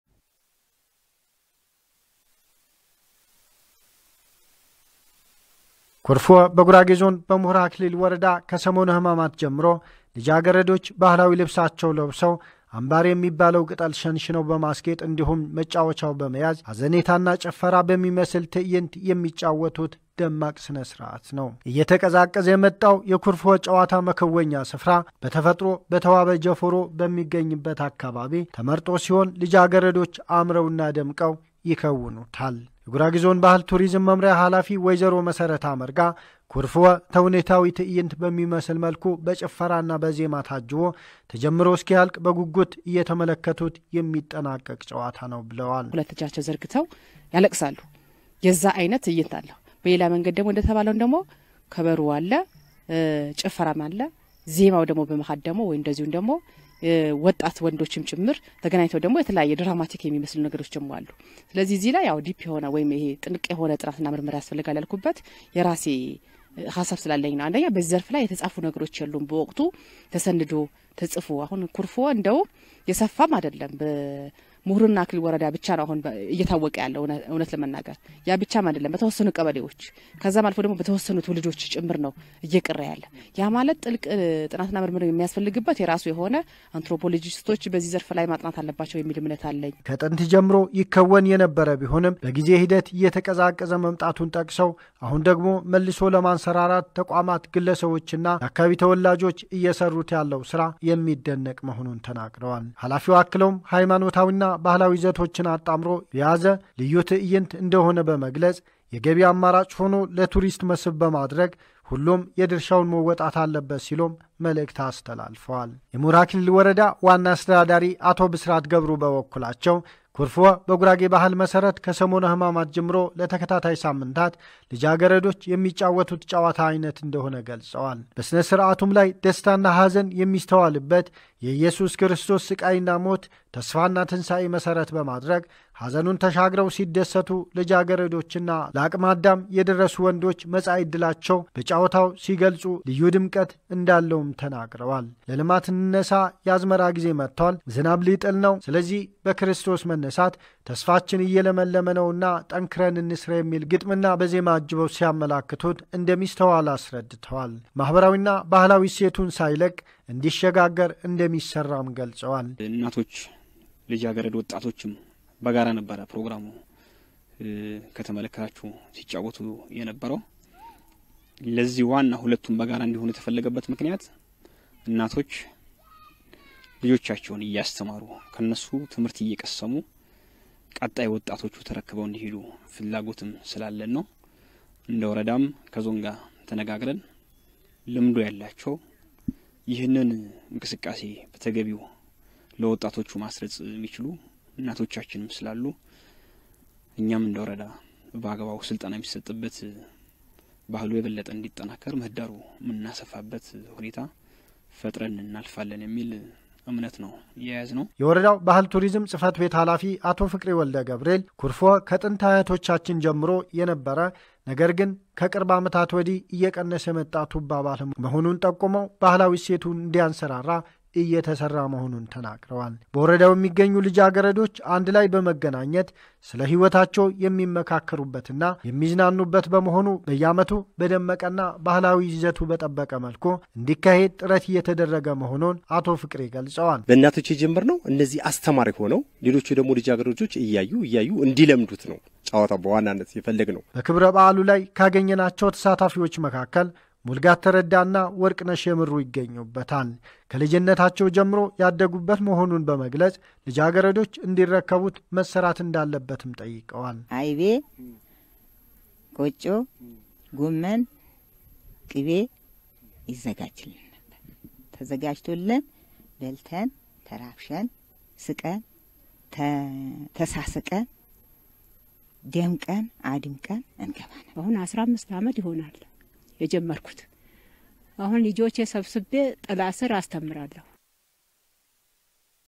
የኩርፍወ ባህላዊ ጨዋታን በማሳደግና ኢኮኖሚያዊ ፋይዳውን በማስተዋወቅ ትውልድ ተሻጋሪ እንዲሆን በትኩረት መስራት እንዳለበት ተገለፀ
ዘጋቢ፡ እንዳልካቸው ደሳለኝ - ከወልቂጤ ጣቢያችን